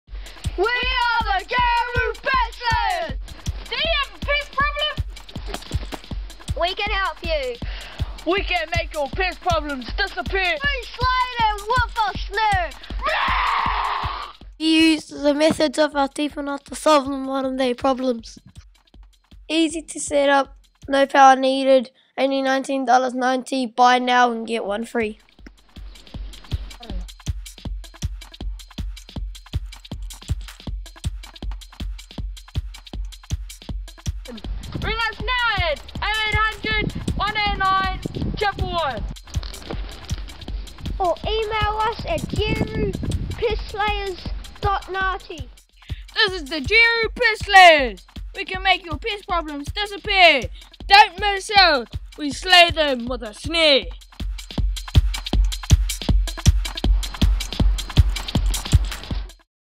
Jeru Pest Slayers
Nati Awards 2014 - "Nati Hangarau: Technology - Yesterday, Today, Tomorrow"

Category: Junior Advertisement Winner
Title: "Jeru Pest Slayers"
School: Hiruharama